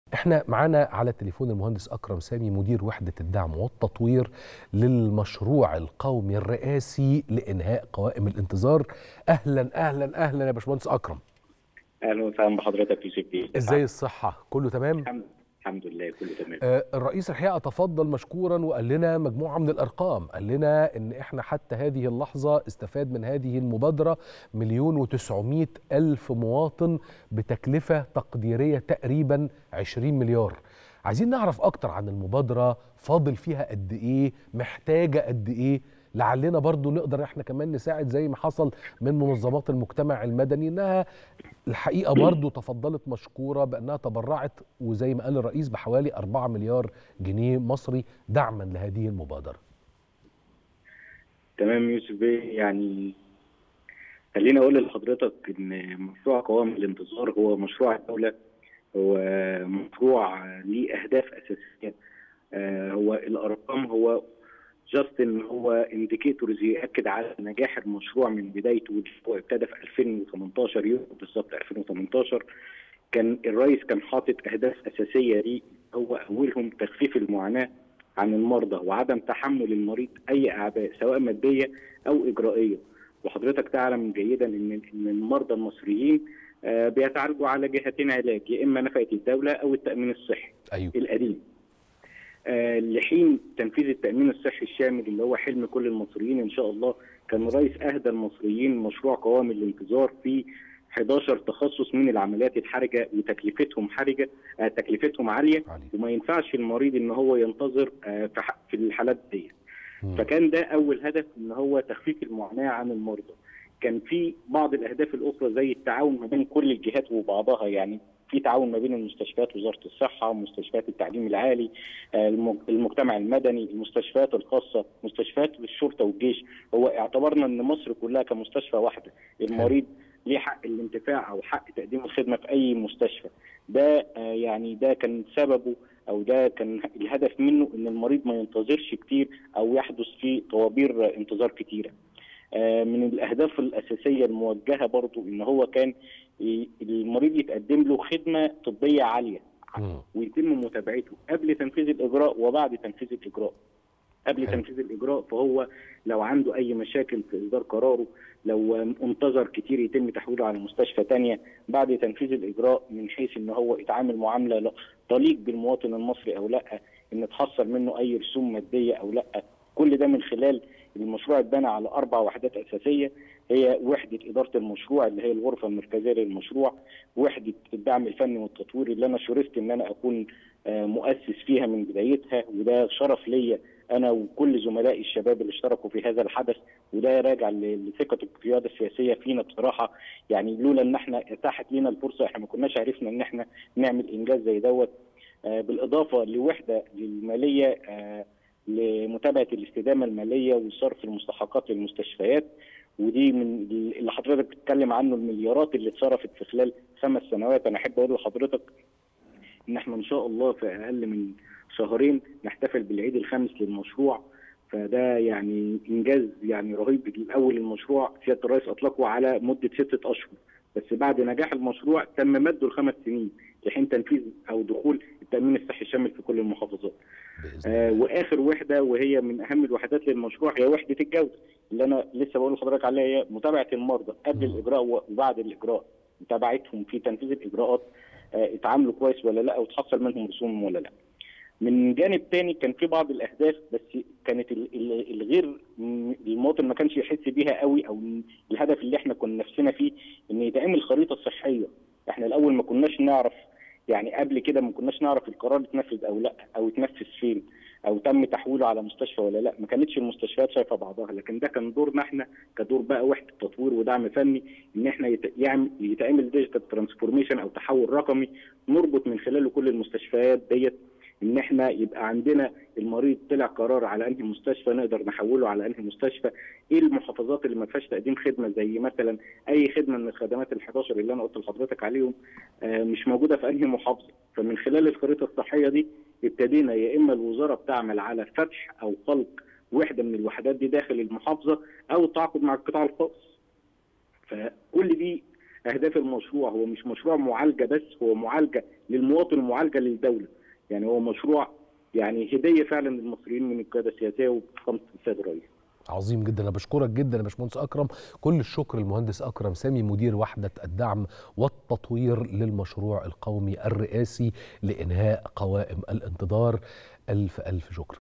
التاسعة | أكرم سامي يوضح أهمية تصريحات الرئيس السيسي خلال فغاليات المؤتمر الطبي الإفريقي الثاني
التاسعة | أكرم سامي مدير وحدة الدعم والتطوير للمشروع القومي يوضح أهمية تصريحات الرئيس السيسي خلال فغاليات المؤتمر الطبي الإفريقي الثاني